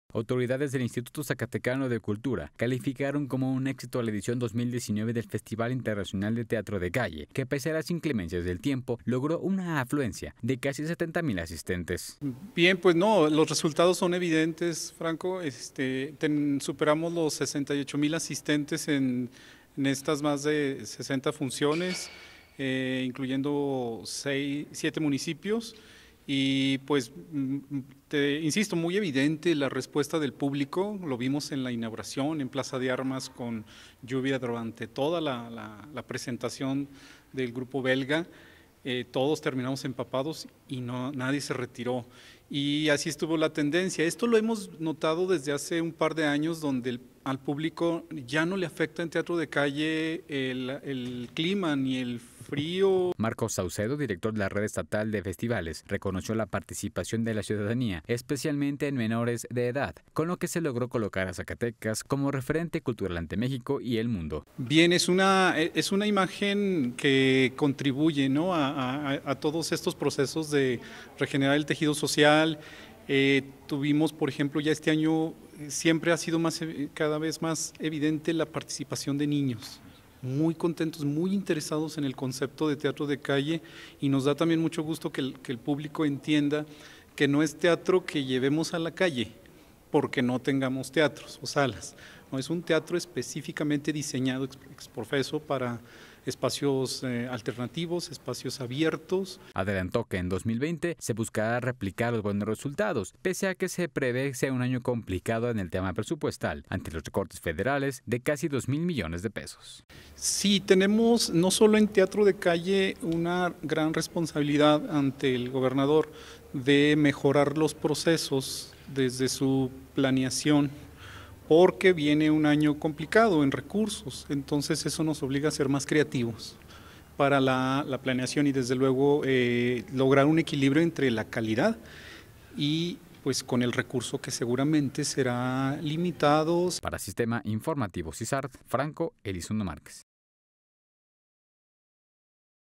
Exitoso, Festival Internacional de Teatro de Calle
Autoridades del Instituto Zacatecano de Cultura (IZC) calificaron como un éxito la edición 2019 del Festival Internacional de Teatro de Calle, pues pese a las inclemencias del tiempo, logró una asistencia de casi 70 mil personas.